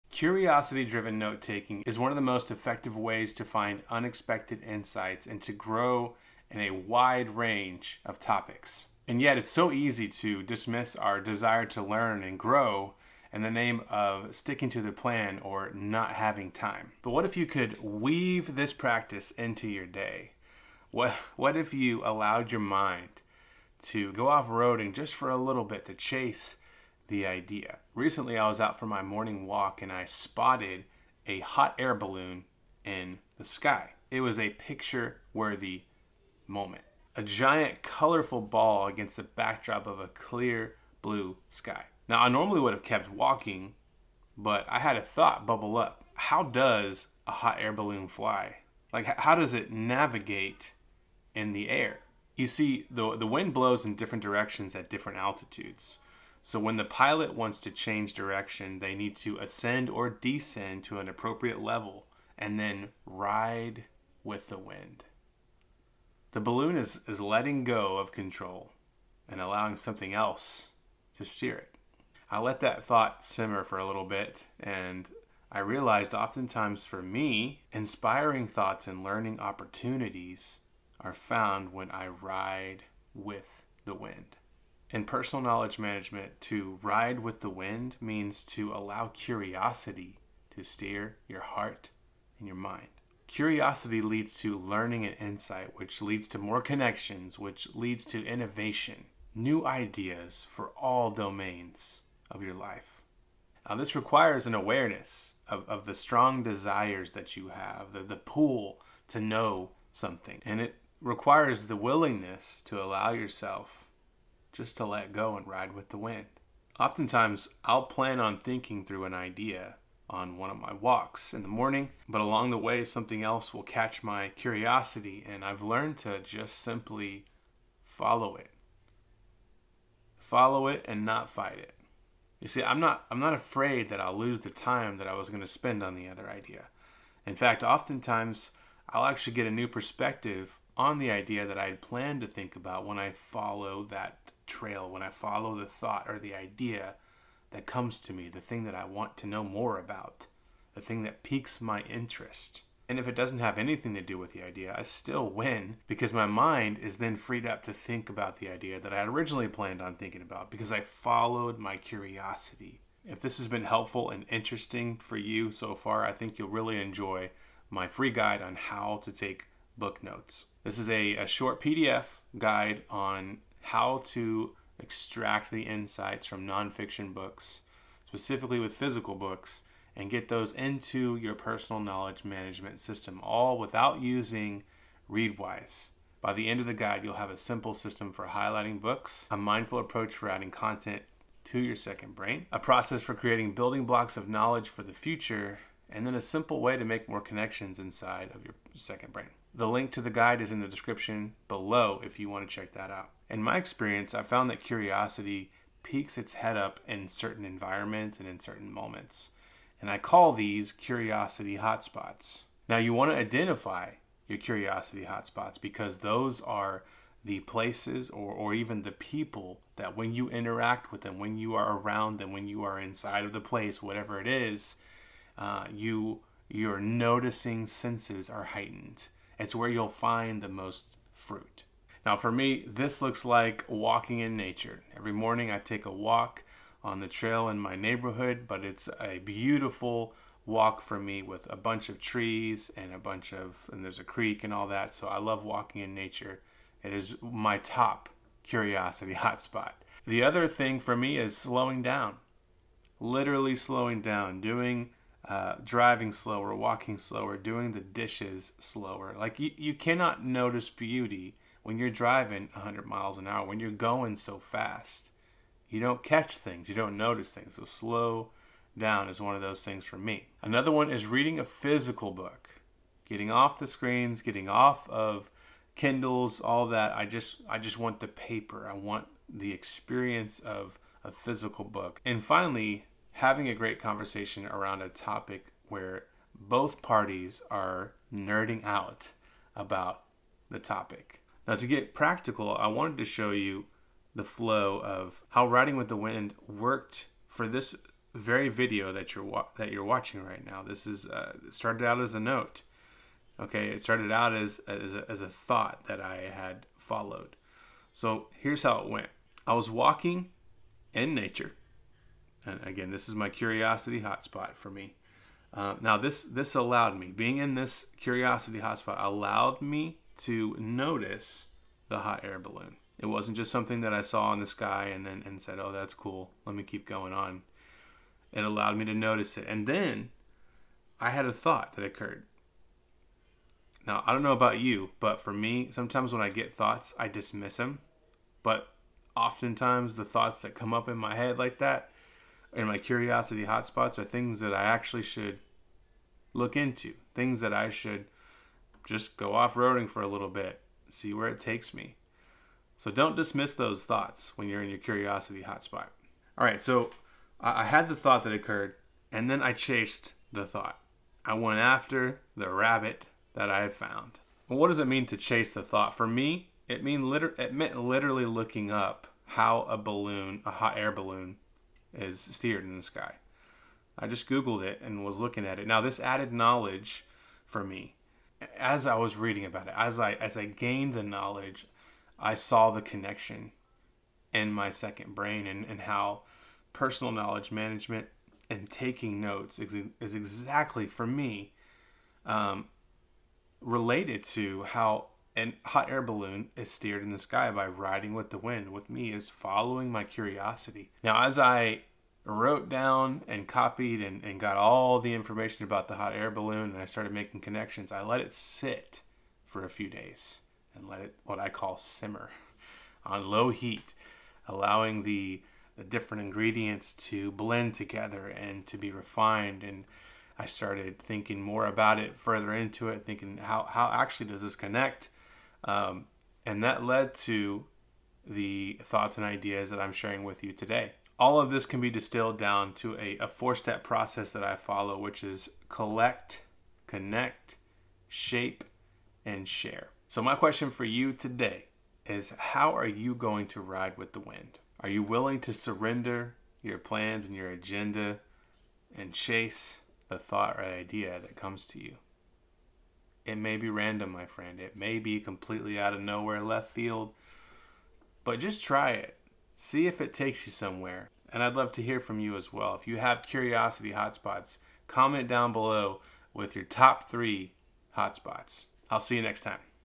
One powerful note-taking method from hot air balloons
VIDEO OVERVIEW
Discover the power of curiosity-driven note-taking with this video! Learn how riding with the wind, inspired by hot air balloons, can lead to unexpected insights and innovation in all aspects of your life. Follow the 4-step process of collecting, connecting, shaping, and sharing to enhance your personal knowledge management (PKM) system (second brain).

CHAPTERS
00:00 Chase the idea
00:27 Ride with the wind
01:24 Curiosity in PKM
02:43 Free Guide: How to take book notes
03:22 Curiosity hotspots
03:50 My hotspots
04:51 Practical example
07:10 4-step PKM process

NEED FOCUSING MUSIC?
I also make lofi/chill music to help you focus and create.